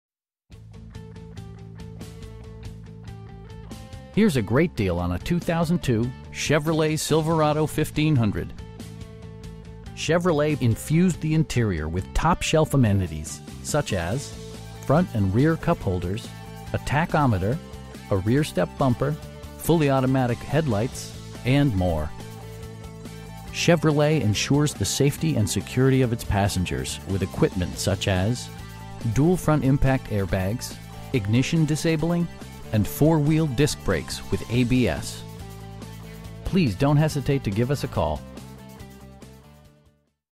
2002 Chevrolet Silverado 1500 near Howell, Michigan
This vehicle is a 2002 Silverado 1500 with about 204K miles and is being powered by a 5. This 2002 Silverado 1500 is located near Howell, Michigan.  3L V-8 cyl engine.  Some of the key features include: abs brakes, am/fm radio and air conditioning.  The burgundy exterior combined with the graphite cloth interior looks great together.  This Truck Extended Cab is being sold as-is without a warranty.  Some more key features on this Silverado 1500 include: power locks, hitch, topper, speakers: 4, bumpers: chrome, tilt steering wheel and front cupholders.  This 2002 Silverado 1500 is a one owner Truck Extended Cab as reported by an AutoCheck Report.  An AutoCheck Report is available upon request.
    Hank Graff Chevrolet Davison
    Address:
    800 N. State St.